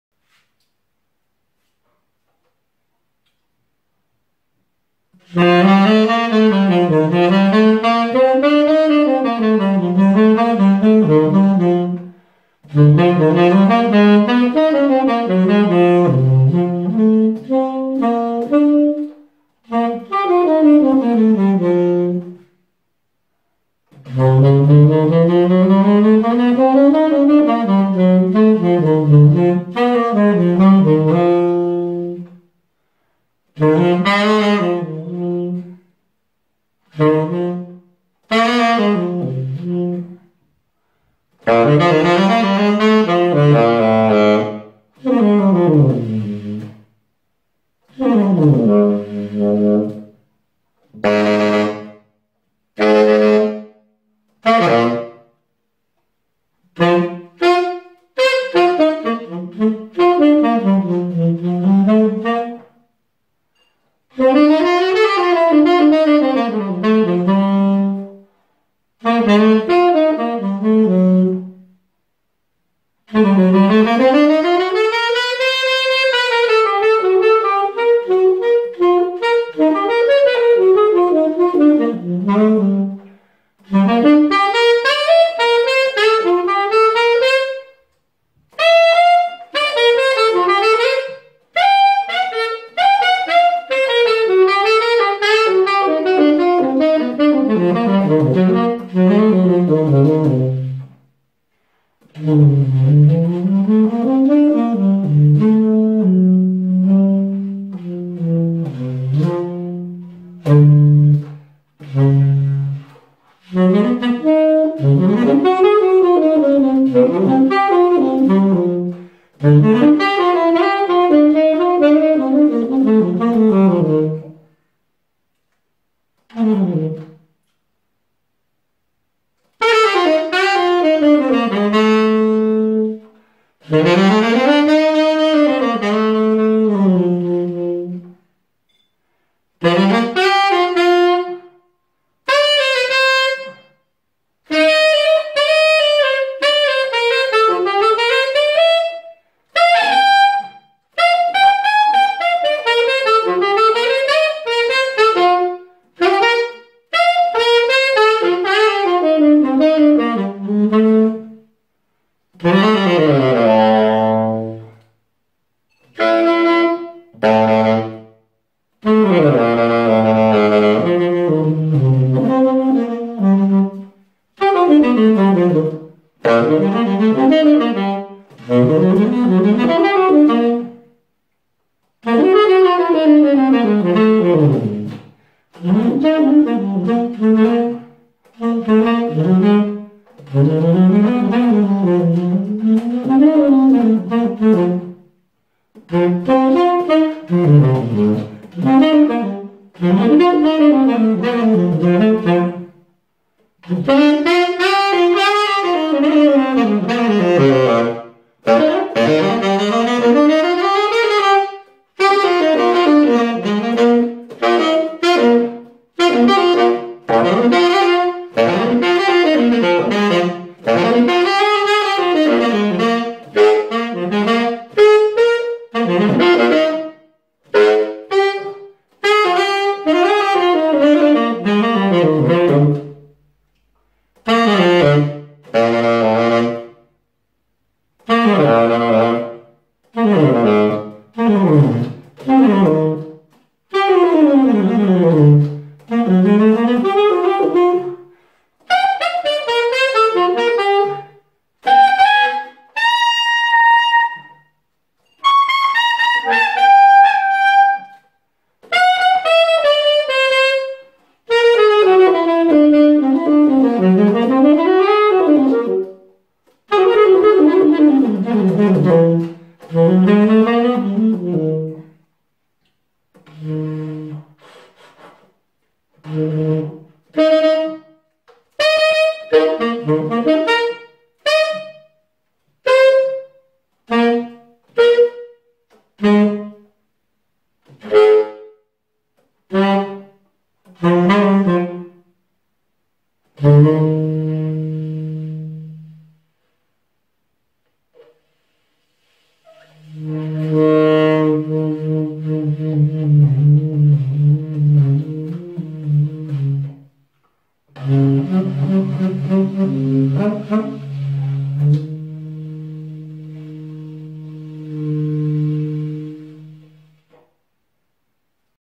20170911_153928 Otto Link STM Florida #7* by Brian Powell - traditional full sound!
Otto Link STM “Florida U.S.A.” #6*@0,105” (by Brian Powell) tenor saxophone mouthpiece
Rico "Jazz Select" #3 Medium filed tenor saxophone reed - pretty used!
Rovner “Light” ligature

Rampone & Cazzani R1 Jazz AGU+ with "X1" neck tenor saxophone

Filmed with a Samsung Galaxy S7, rear camera. No editing: raw audio!
Recorded with a Kel Audio HM-2D into the smartphone mic input.

The lack of sustain/reverb is because of the AGC (auto gain control) of the mobile phone.

The mouthpiece is for sale. NOW SOLD!